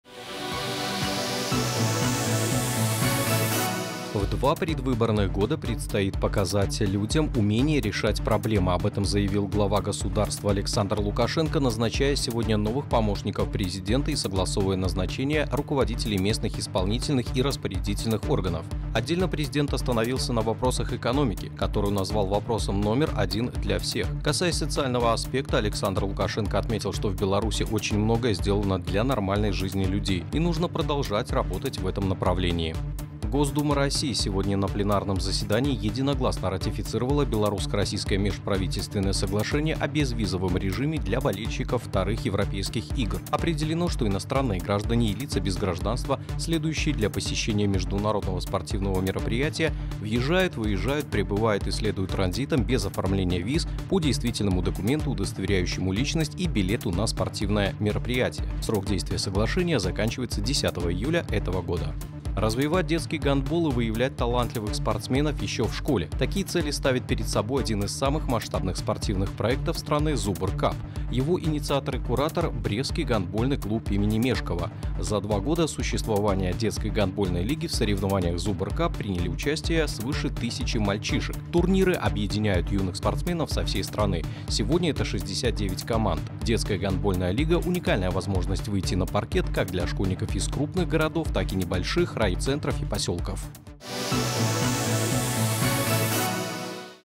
Новости Беларуси 12.02. 2019 16-00
Александр Лукашенко назначил новых помощников и согласовывал назначение руководителей местных исполнительных и распорядительных органов
Госдума России ратифицировала белорусско-российское соглашение о безвизовом режиме для болельщиков ІІ Европейских игр
В Беларуси реализуется масштабный спортивный проект ZubrCup по развитию гандбола